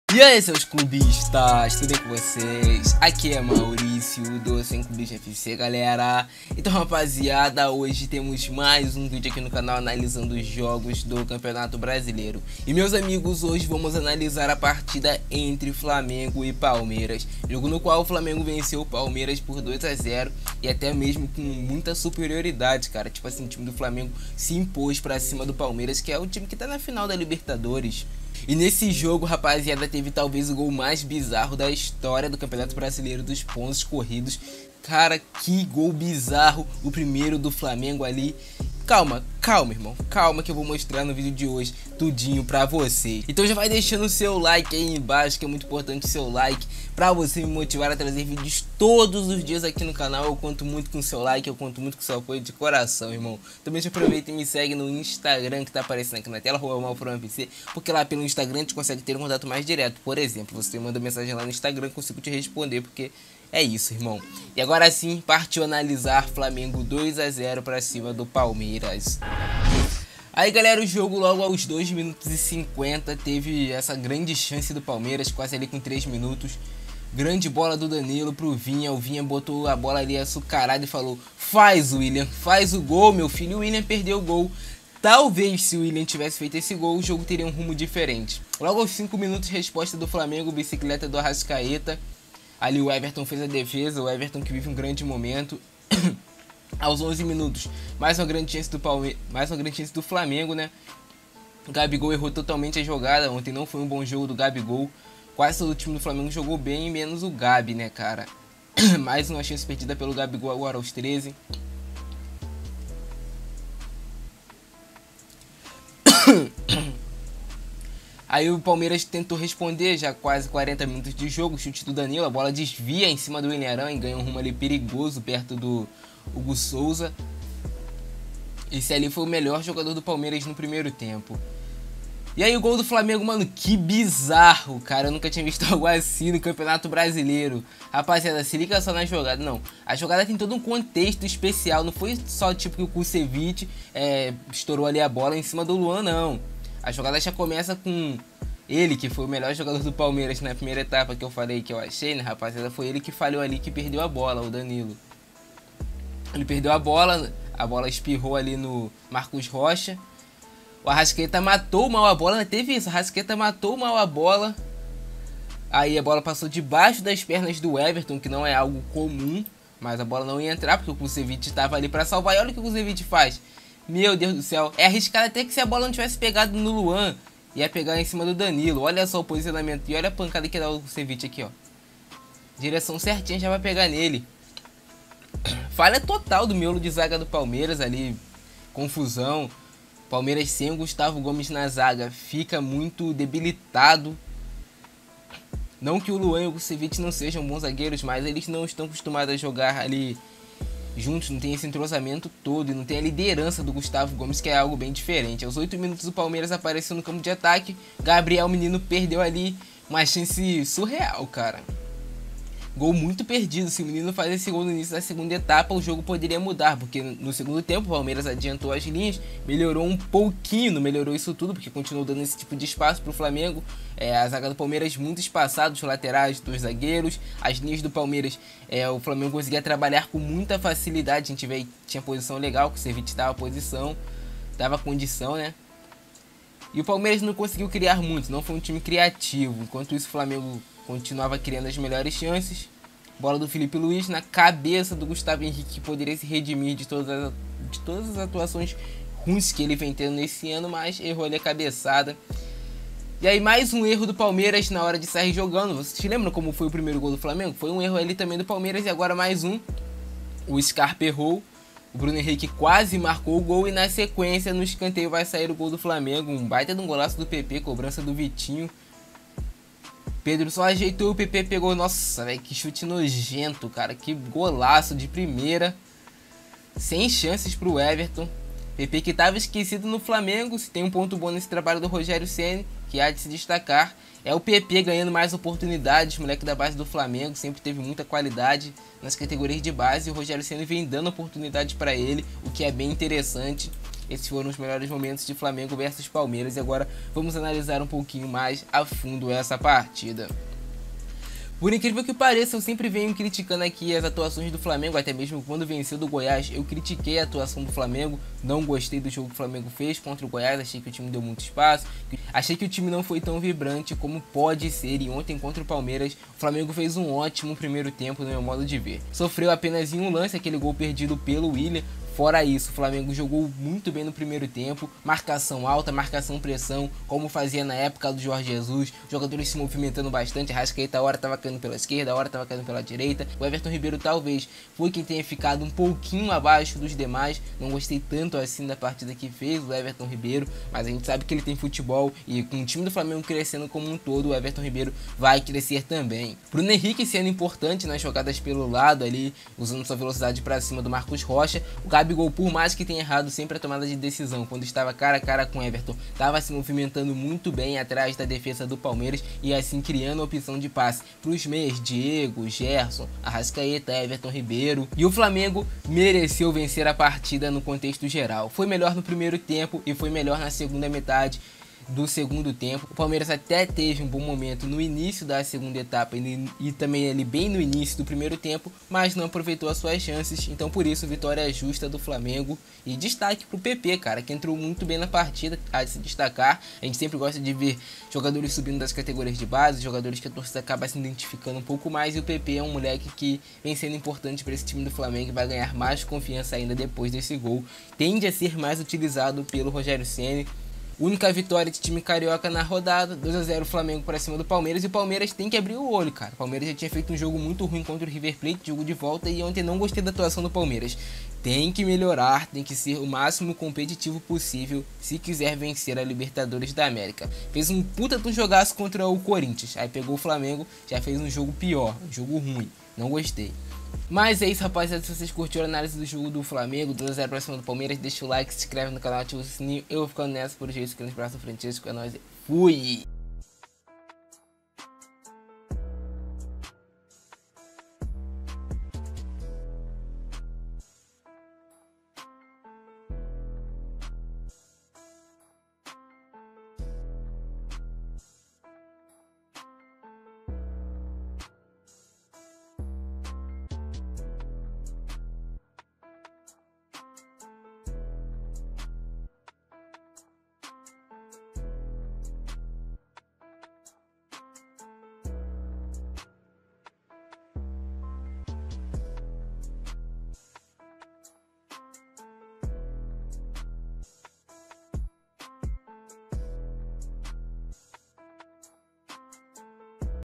O GOL CONTRA MAIS BIZARRO DA HISTORIA DO BRASILEIRÃO! - ANALISE FLAMENGO X PALMEIRAS
Então galera hoje vamos ver o gol mais bizarro da historia do campeonato brasileiro, o gol mais bizarro da historia do brasileirão, esse gol bizarro aconteceu na partida entre flamengo x palmeiras, jogo que o flamengo venceu por 2x0, vamos também analisar a partida entre flamengo x palmeiras.

LOJA